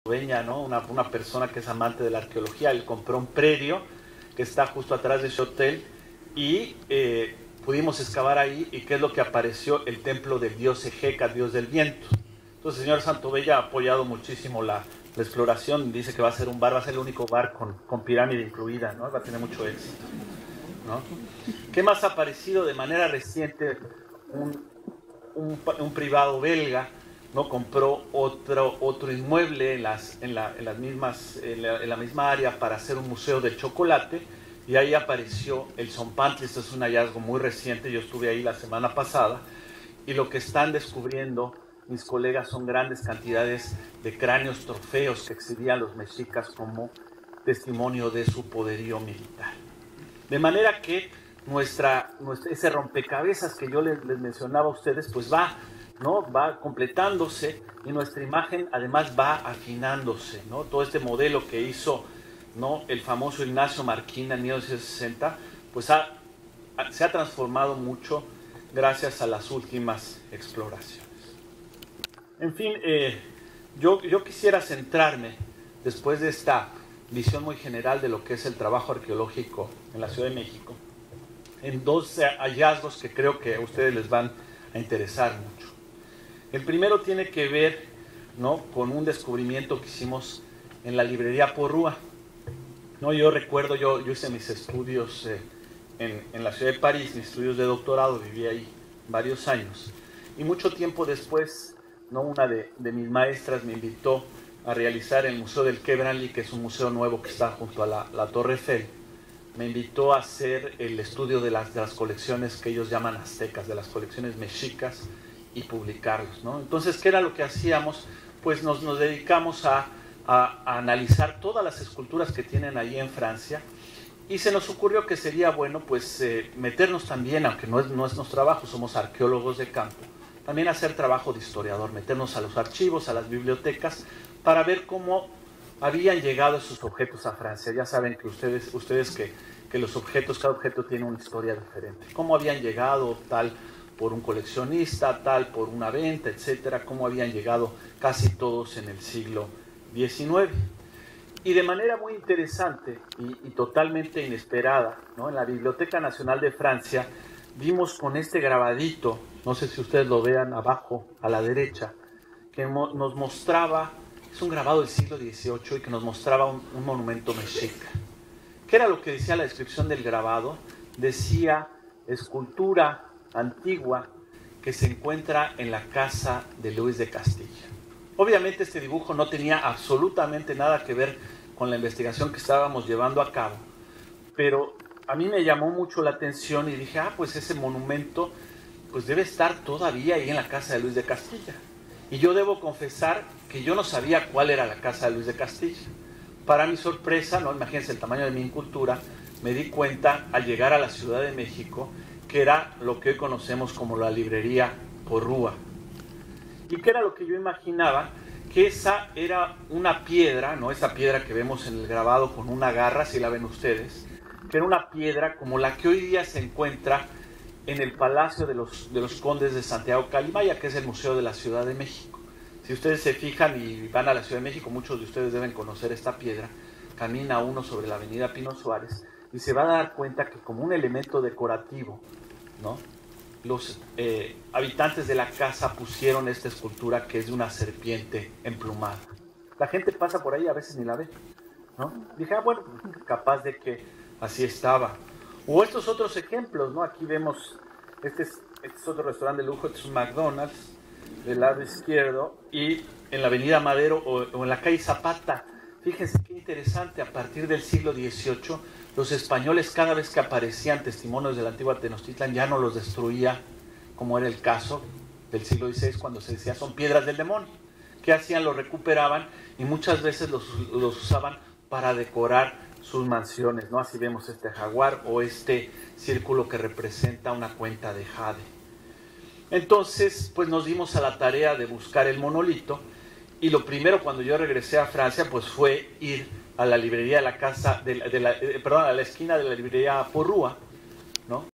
Tenochtitlan (V-2): Arqueología en el corazón de la Ciudad de México. México a través de la Cultura.
V2: Tenochtitlan... Arqueología en el corazón de la Ciudad de México.
México a través de la Cultura.
Impartió: Dr. Leonardo López Luján.

26 de Enero 2017
Sala Carlos Monsivais del Centro Cultural Tijuana.